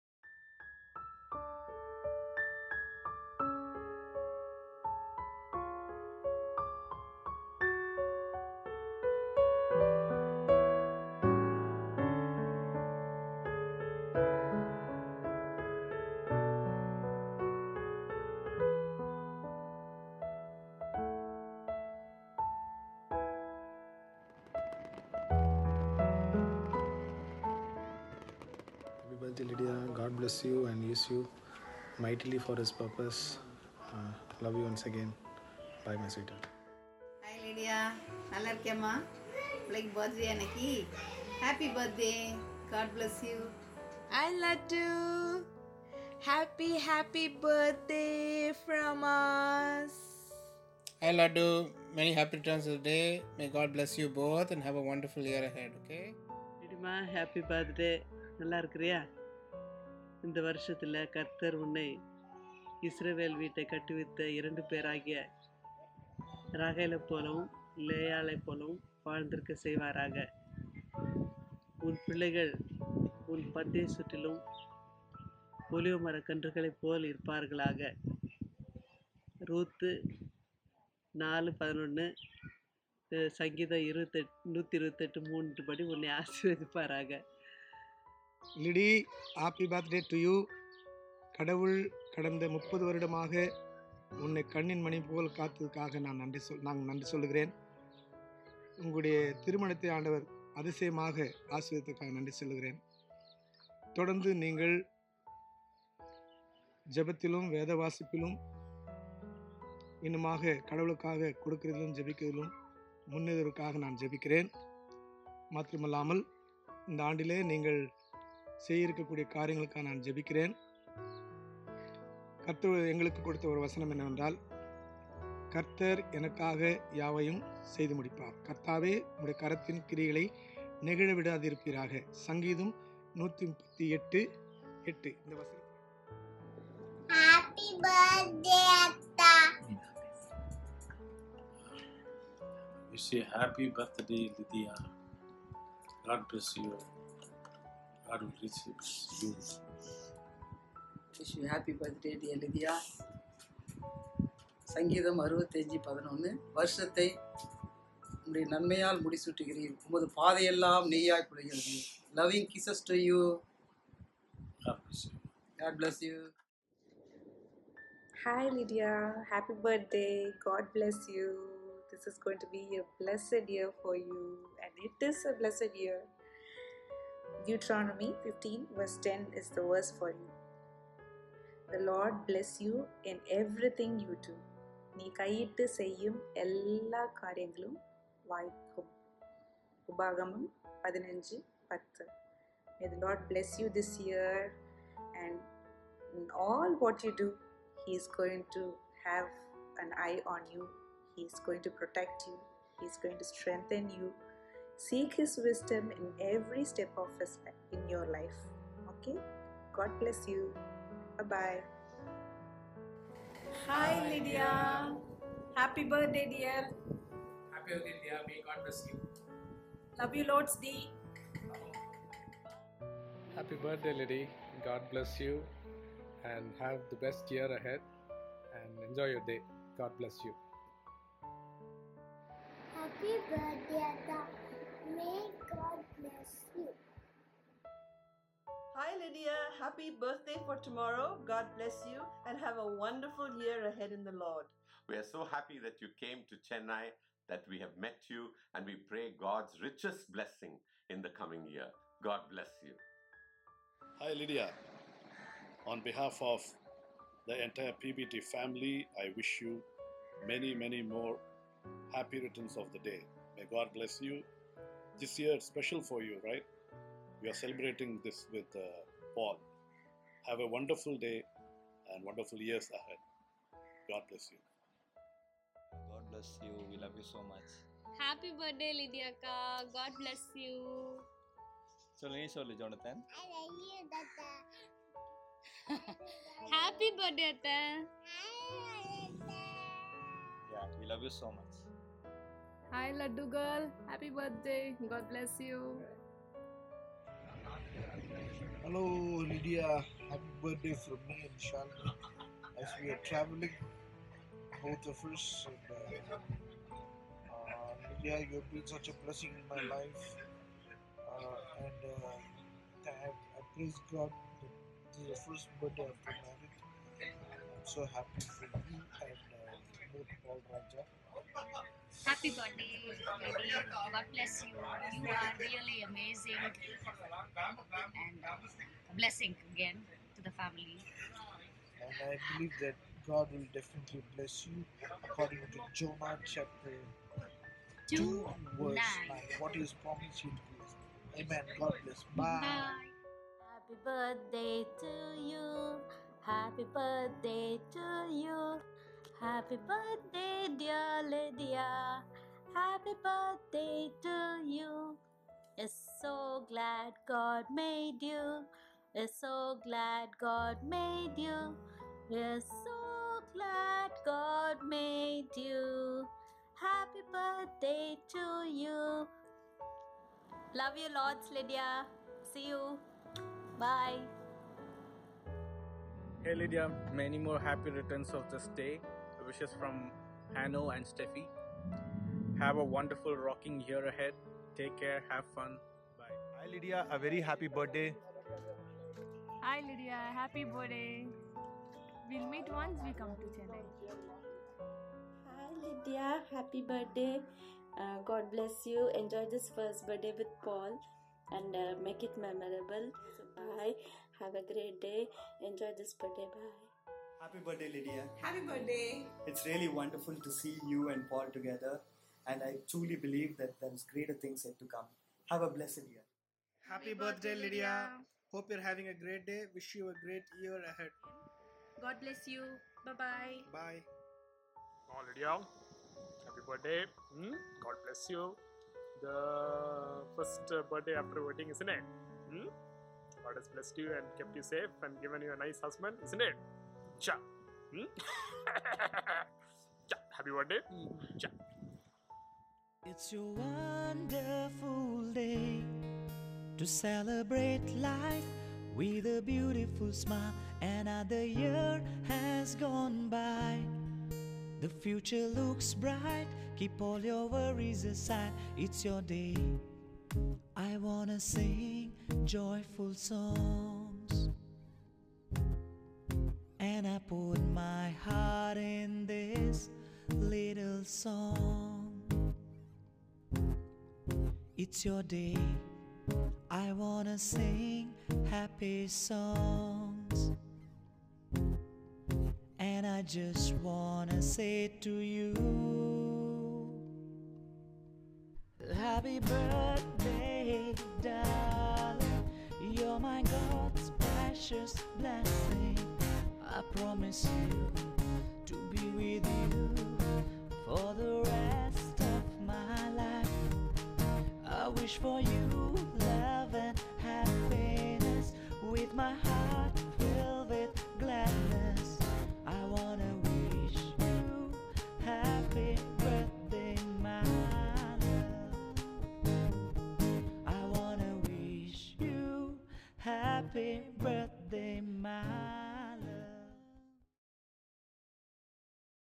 BIRTHDAY WISHES FOR MY LOVE 'LYDIA'
Wishes from all the loved ones for Lydia's special birthday by Paul.

Edited by Christopher Marandi